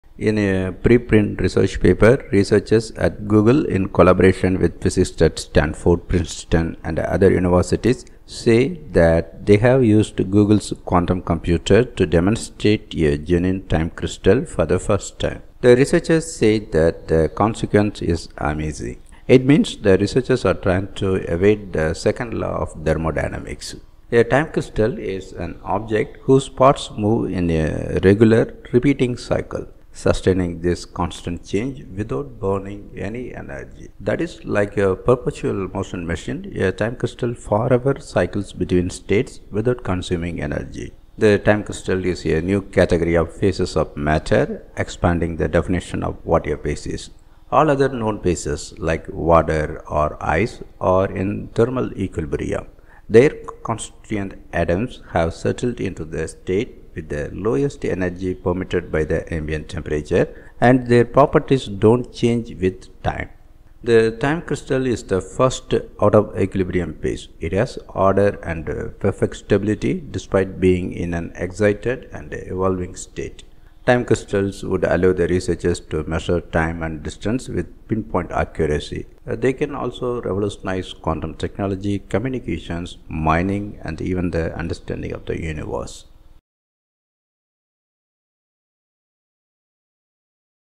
Google's 'time crystals’ will be the new phase of matter
In a preprint research paper, researchers at Google in collaboration with physicists at Stanford, Princeton and other universities say that they have used Google's quantum computer to demonstrate a genuine "time crystal" for the first time.

The researchers say that the consequence is amazing: It means the researchers are trying to evade the second law of thermodynamics.

Research paper (preprint): Observation of Time-Crystalline Eigenstate Order on a Quantum Processor